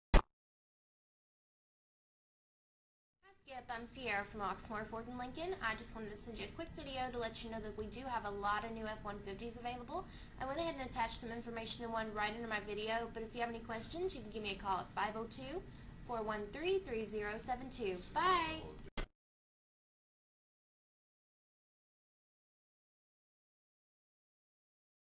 2013 Ford F-150 for Skip in Louisville Ky 40211
Skip was looking at a 2013 Ford F-150 here at Oxmoor Ford Lincoln in Louisville KY. Ciera sent over this personal video to answer their questions and introduce themselves. They received a quote and additional information on the Ford F-150.